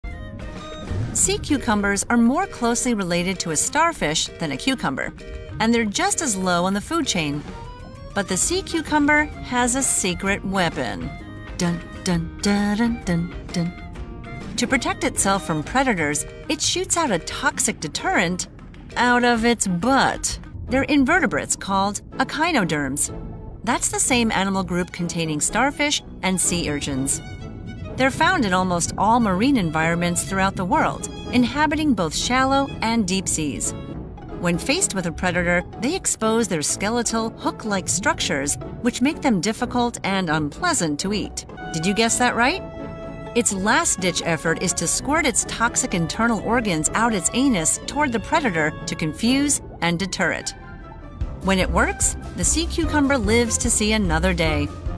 THE SEA CUCUMBER SECRET WEAPON sea cucumber
Sea cucumbers are marine invertebrates found in oceans worldwide, residing on the ocean floor. Despite their cucumber-like appearance, they're echinoderms, related to sea stars and sea urchins. These fascinating creatures play vital roles in ocean ecosystems, recycling nutrients and aerating sediments as they feed on detritus. They possess a unique defense mechanism, expelling their internal organs as a distraction to predators, which later regenerate. Sea cucumbers have economic importance, particularly in Asian cuisine and traditional medicine, where they're believed to have various health benefits.